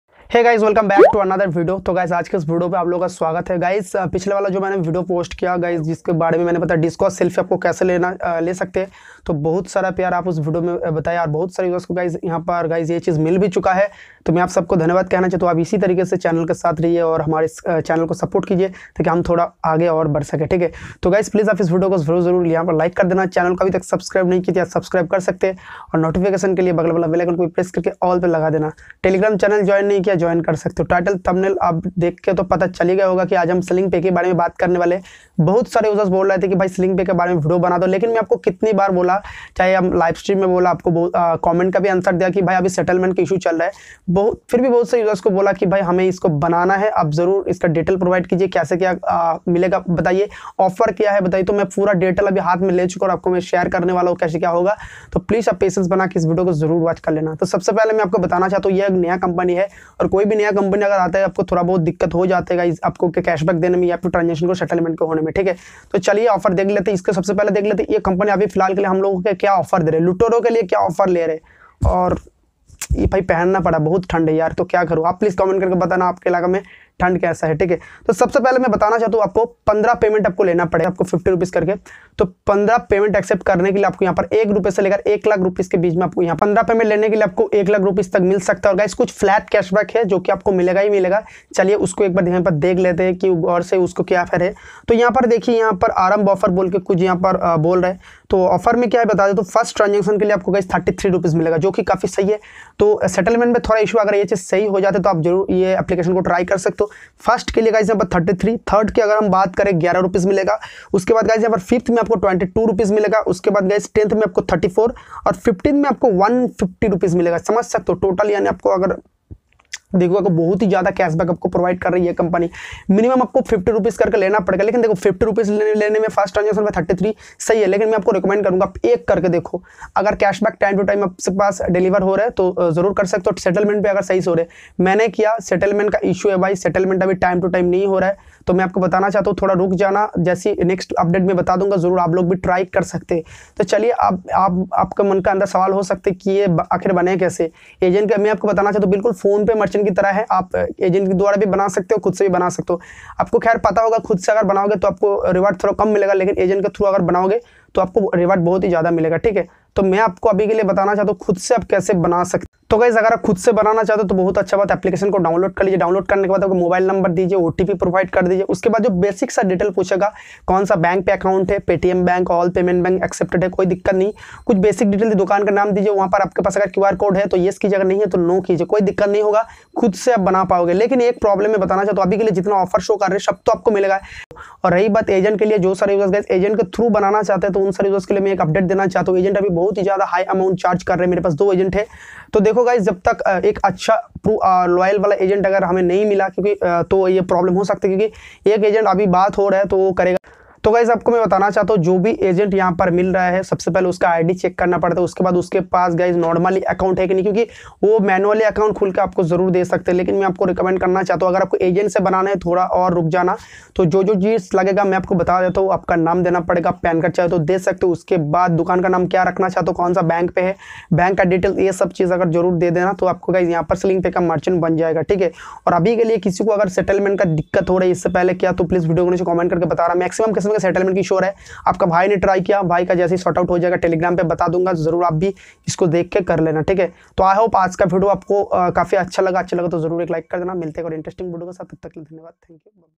how to create slingpe merchant, slingpe merchant kaise banaye, create slingpe merchant earn 100000
Hello guys aj ki is video me apko bataunga sling pe merchant ki bareme.

Agent Ka no teligram pr dunga.